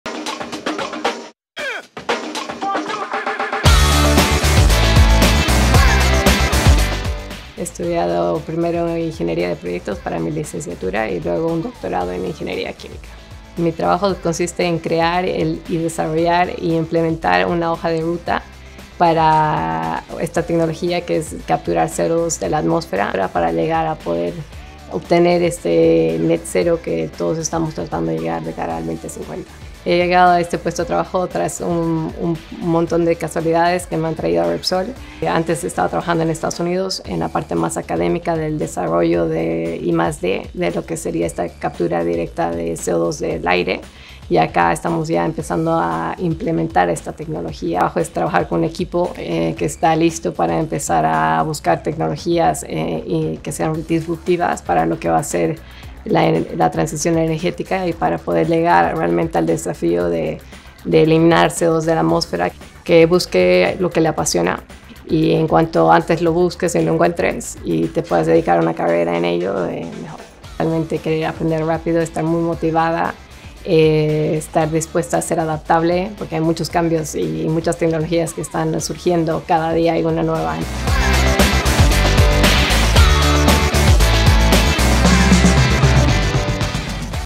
¿Qué hay que hacer para ser... Científico Senior en RepsolTechLab?
¿Capturar el CO₂ de la atmósfera para convertirlo en un aliado? ¡Es posible!
Nuestra compañera Mica, Científico Senior en RepsolTechLab, ya está desarrollando esta innovadora tecnología.

Si quieres saber qué hay que hacer para ser científico dentro de nuestra compañía y participar en proyectos que aporten al futuro de nuestra sociedad, ¡no te pierdas este vídeo!